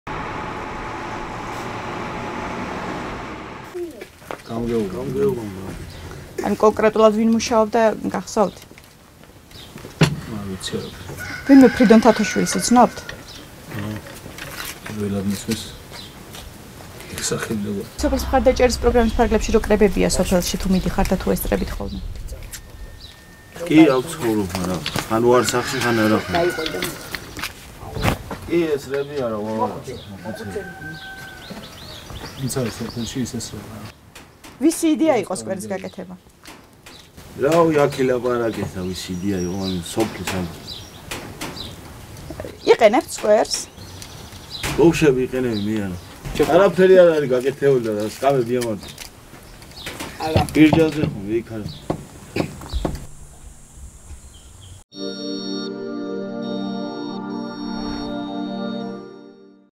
ბიუჯეტი სოფლისთვის და „ოცნების“ მხარდამჭერებისთვის - ვის მისცა ადიგენის მერიამ 1.5 მლნ ლარი
ადიგენის მუნიციპალიტეტის სოფელ პატარა სმადაში 2022 წელს „სოფლის მხარდაჭერის პროგრამის“ ფარგლებში პრიორიტეტად სკვერის მშენებლობა შეირჩა. სატენდერო დოკუმენტაციის მიხედვით, სამუშაოები გამარტივებული შესყიდვით  ფრიდონ თათოშვილმა აიღო. ამასთან, ამ ერთი სკვერის მშენებლობა პრიორიტეტი იყო, როგორც 2022 წელს, ასევე 2023 წელსაც.